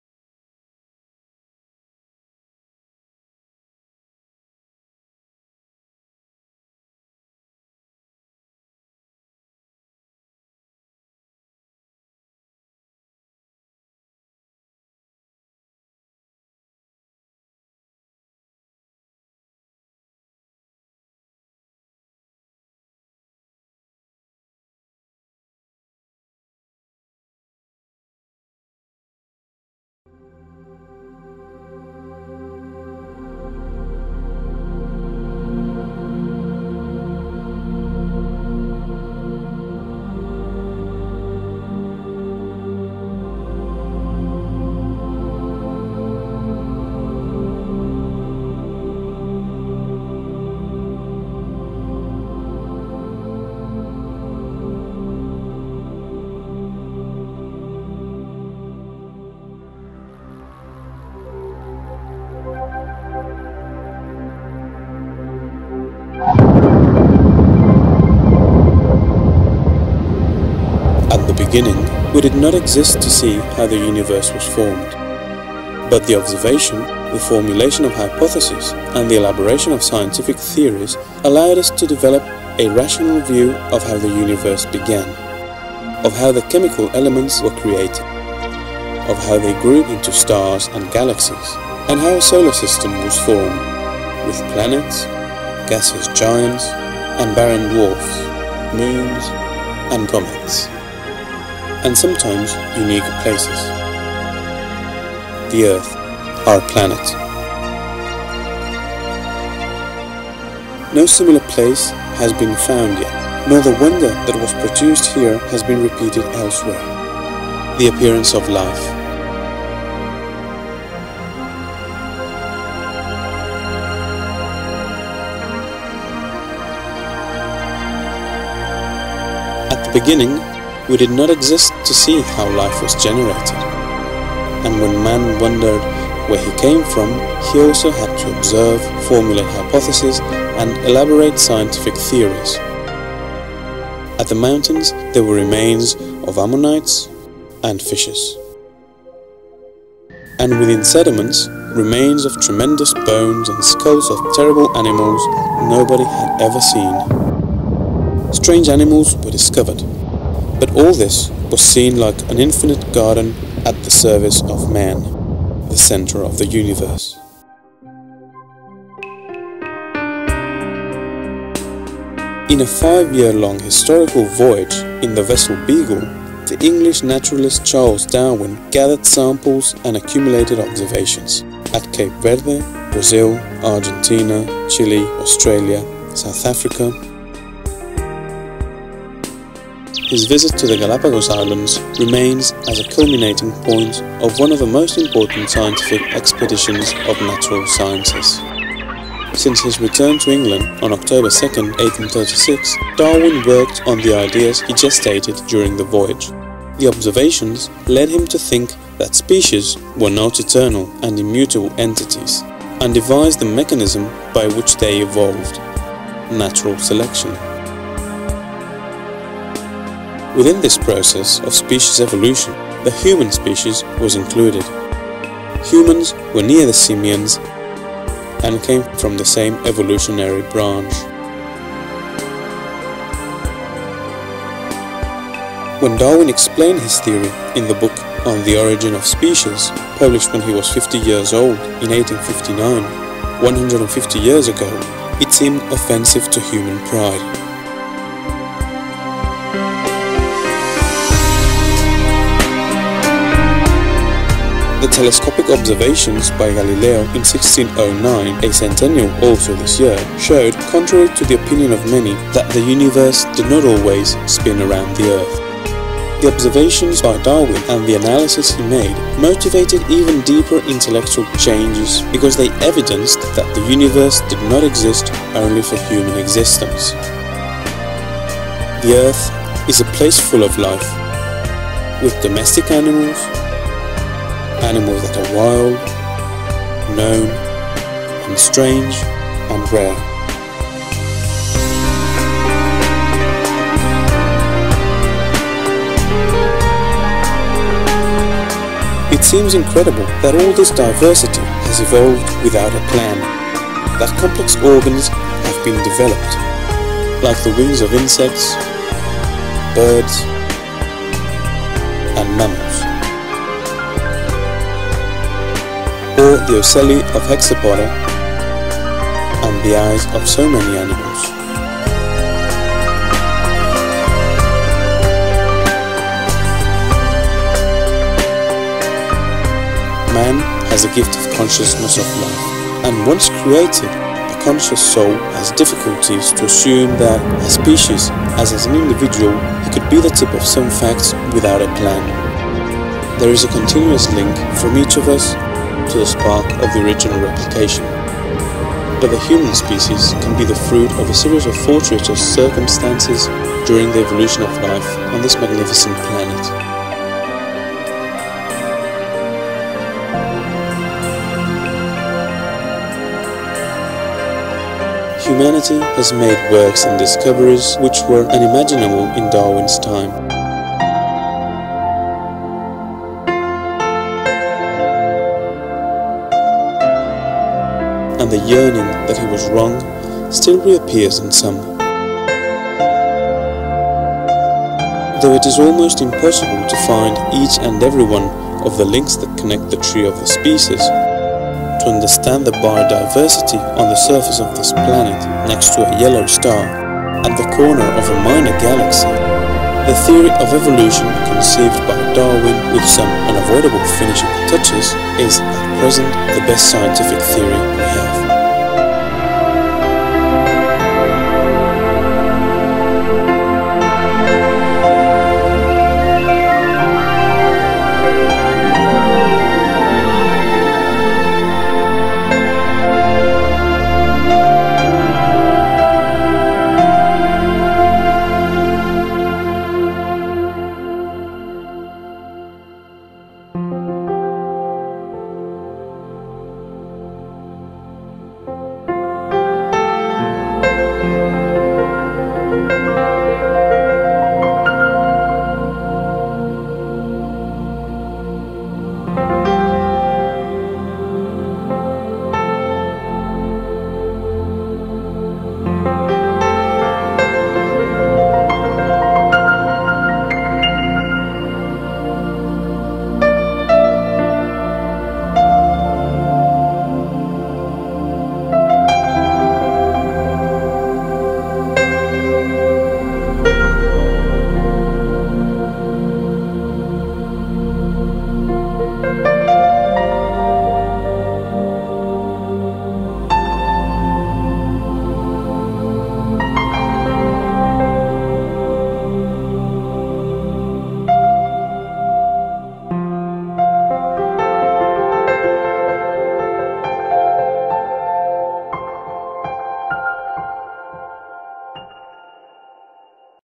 The best one we have
To celebrate Darwin Year 2009 a DVD has been produce to introduce the impact of Darwins theory of evolution. The script and production is by Antoni Amengual, professor at the Universitat de les Illes Balears. The DVD is published with a license Creative Commons. If the presentation is shown in public, we would appreciate an e-mail to notify this use. The information is only for statistical purposes. The DVD is also available in Spanish and Catalan and can be downloaded with DVD quality